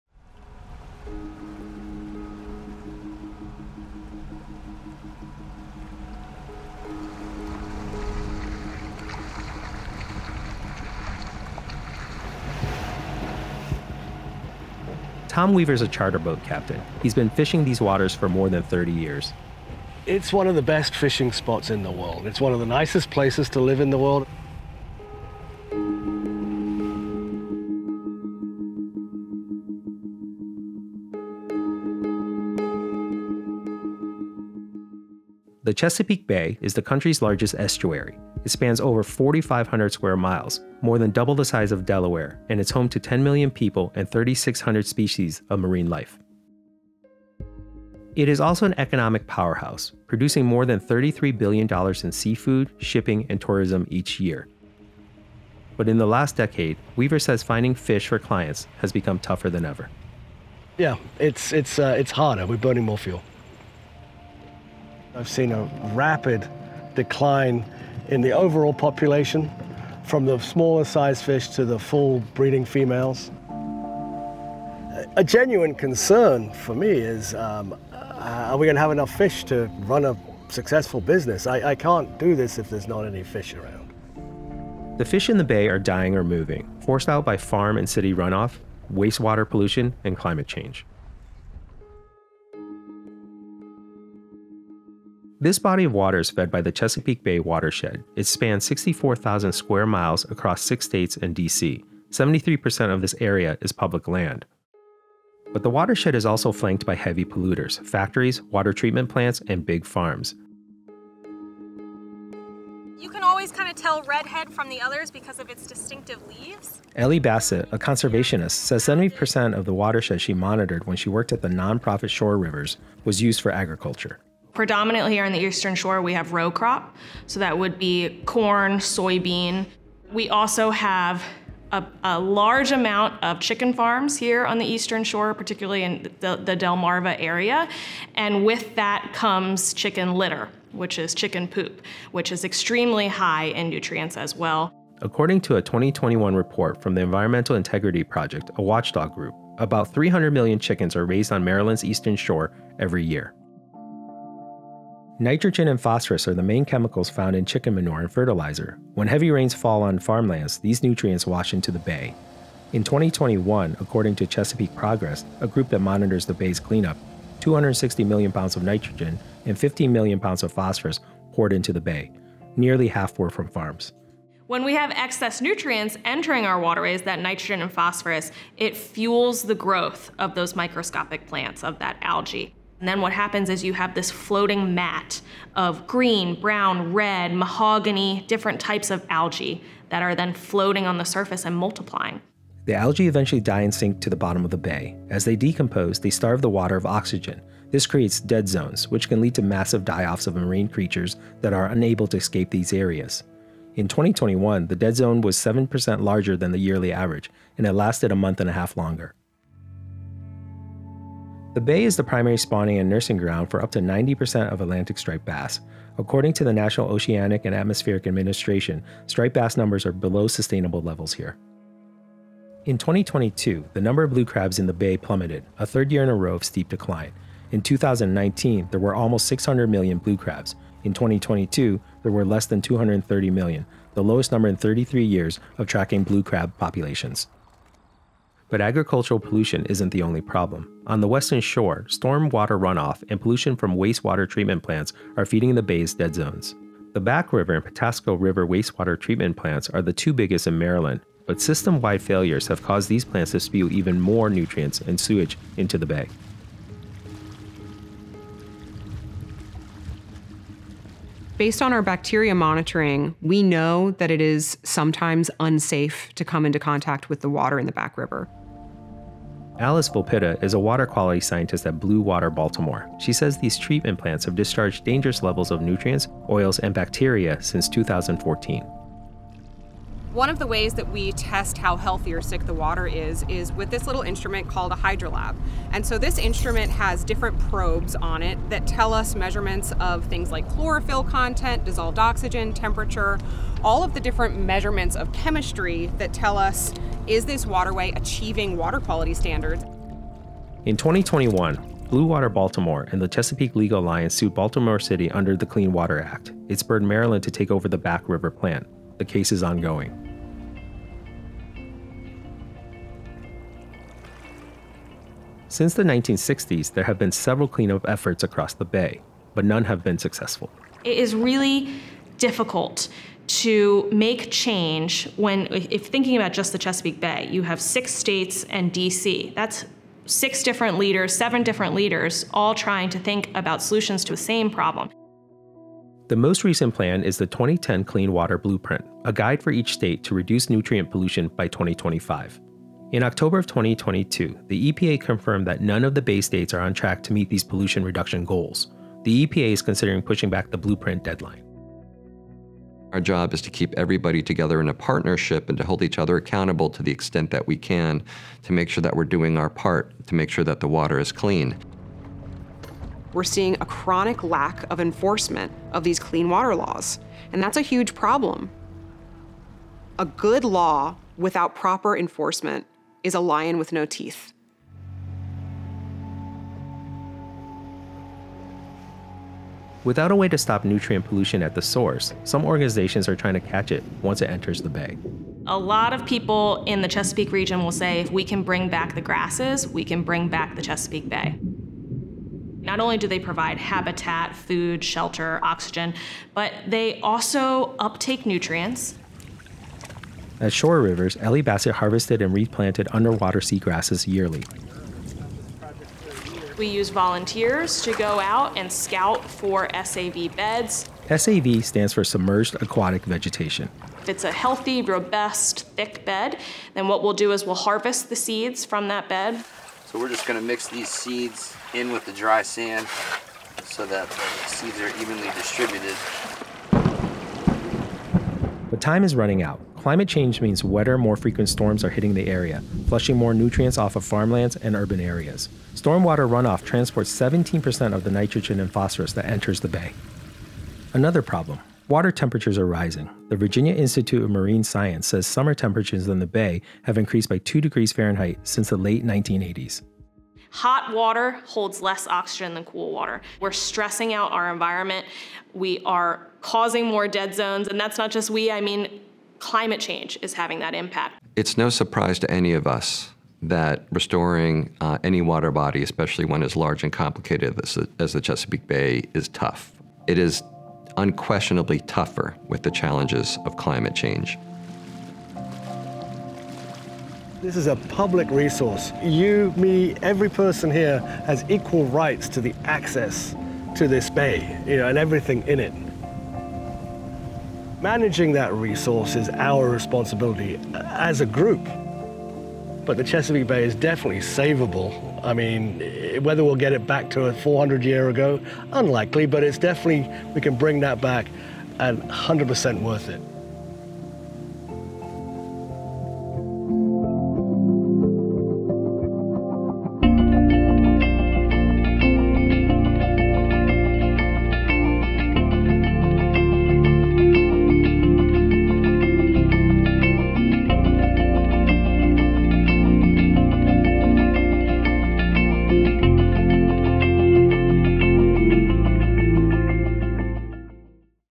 Will the Chesapeake Become a Dead Zone?
In the 45,000 square mile Chesapeake Bay, the country’s largest estuary, nitrogen and phosphorus from wastewater treatment plants, and urban and agricultural run-off is continuously suffocating marine life.

“What happens in the Chesapeake Bay is not only important to our residents, but it also impacts seafood industries, recreation and commercial anglers all along the Atlantic Coast,” says Allison Colden, a senior fisheries scientist at the Chesapeake Bay Foundation, an independent conservation organization.

Despite decades of clean up efforts, and evolving regulations from the Environmental Protection Agency, the bay remains in a critical state. To make matters worse, climate change is compounding the region’s problems. Increased rainfall, which flushes more nutrients into the bay, and warming water temperatures is making it harder to reverse the damage already done to the bay.